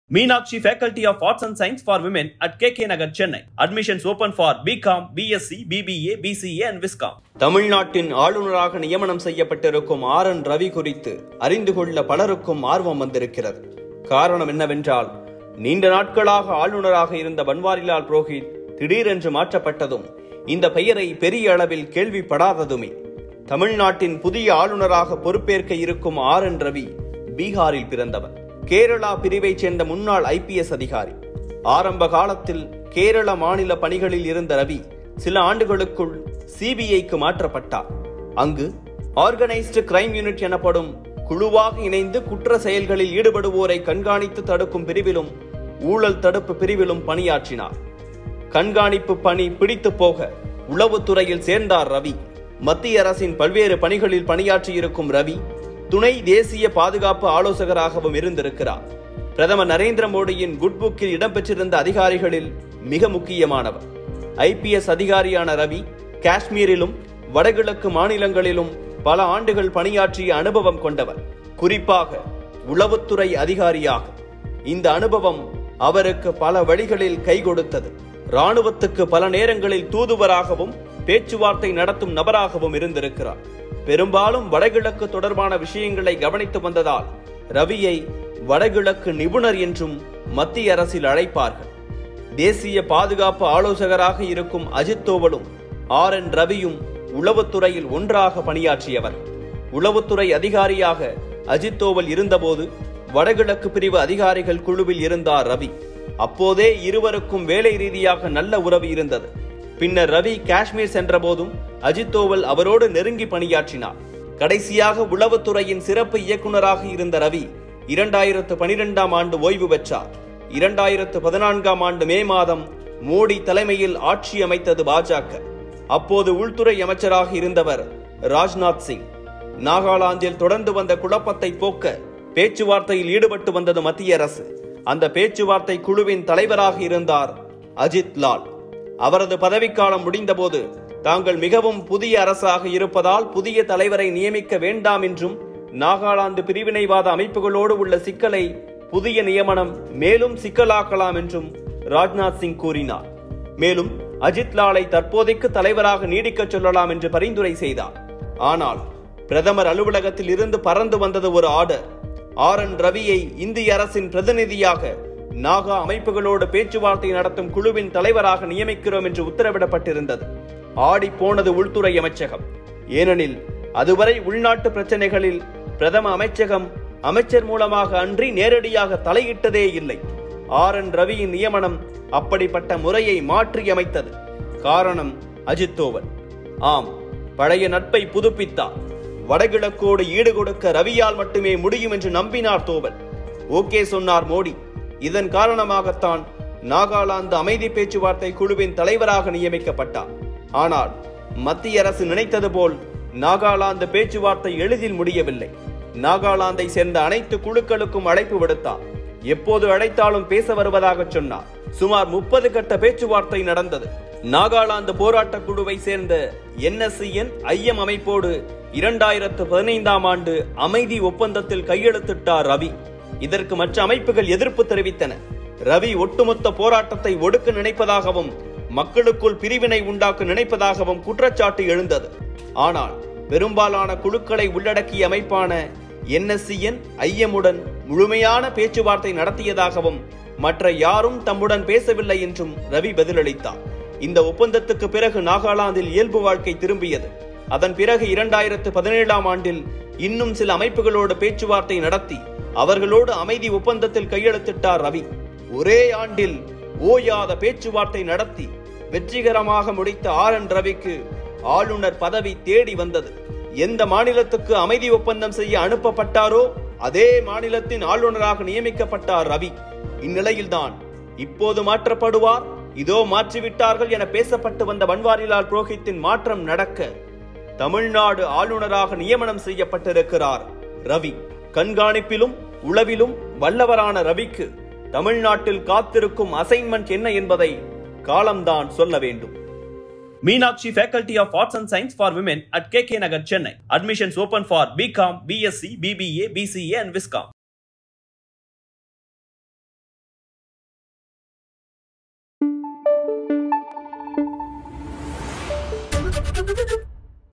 மோடியின் நம்பிக்கை.. தோவலின் தோழன்.. யார் இந்த ஆளுநர் RN ரவி? RN Ravi | Tamil | Tamilnadu Governor
தமிழ்நாடு ஆளுநராக நியமிக்கப்பட்டுள்ள ஆர்.என்.ரவி குறித்து அறிந்து கொள்ள பலருக்கும் ஆர்வம் வந்திருக்கிறது. காரணம் என்னவென்றால் நீண்ட நாட்களாக இருந்த பன்வாரிலால் புரோஹித் திடீரென மாற்றப்பட்டதும் , இந்த பெயரை பெரிய அளவில் கேள்விப்படாததுமே.

தமிழ்நாட்டின் புதிய ஆளுநராக பொறுப்பேற்கவிருக்கும் ஆர்.என்.ரவி பீகாரில் பிறந்தவர். கேரளா பிரிவைச் சேர்ந்த முன்னாள் ஐபிஎஸ் அதிகாரி. ஆரம்ப காலத்தில் கேரள மாநில பணிகளில் இருந்த ரவி, சில ஆண்டுகளுக்குள் சிபிஐக்கு மாற்றப்பட்டார். அங்கு Oraganised Crime unit எனப்படும் குழுவாக இணைந்து குற்ற செயல்களில் ஈடுபடுவோரை கண்காணித்து தடுக்கும் பிரிவிலும் ஊழல் தடுப்பு பிரிவிலும் பணியாற்றினார். கண்காணிப்பு பணி பிடித்துப் போக, உளவுத்துறையில் சேர்ந்தார் ரவி. மத்திய அரசின் பல்வேறு பணிகளில் பணியாற்றியுள்ள ரவி, துணை தேசிய பாதுகாப்பு ஆலோசகராகவும் இருந்துள்ளார். பிரதமர் நரேந்திர மோடியின் Good Book ல் இடம்பெற்றிருந்த அதிகாரிகளில் மிக முக்கியமானவர்.

CREDITS:
Script: Manoj Prabakar
Editing: Vivekanandan
Voice Over: Arunmozhivarman